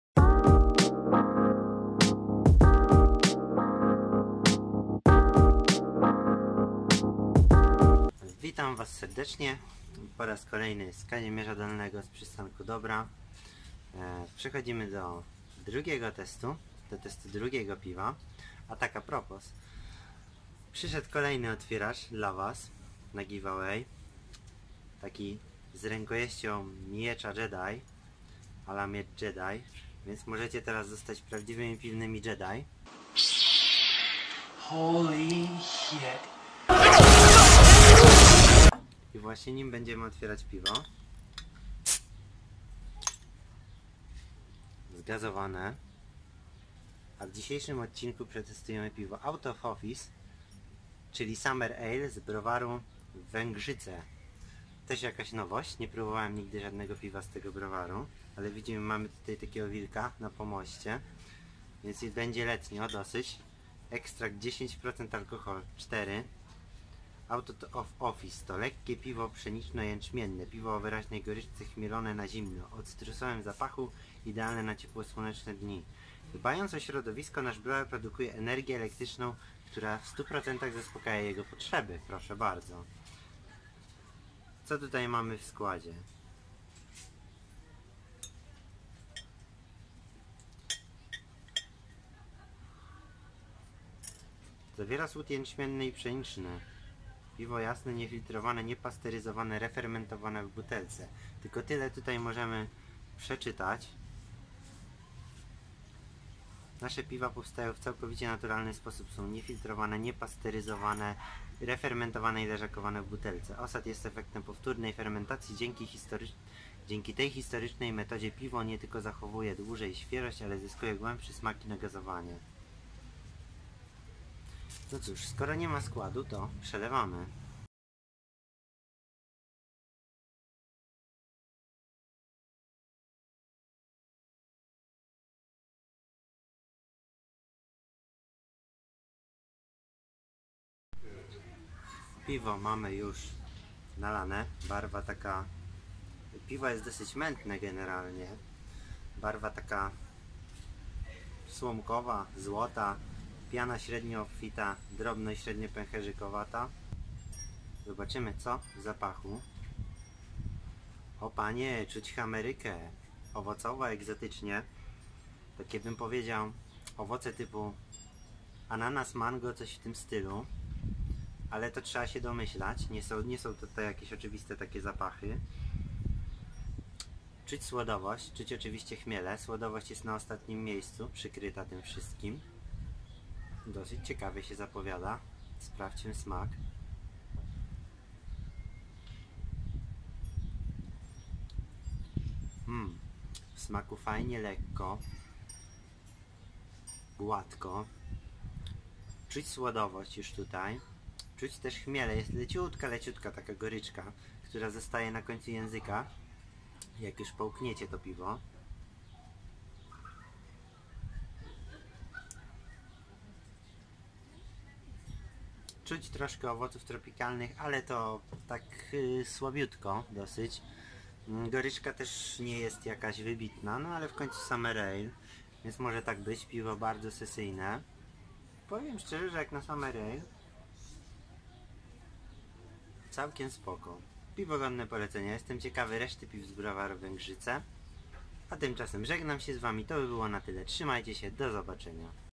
Browar Węgrzce Wielkie - Out of office
Wrzucam zaległy odcinek z sierpniowej wizyty w Przystanku Dobra w Kazimierzu Dolnym. A tymczasem montuje dla was spory materiał!!:)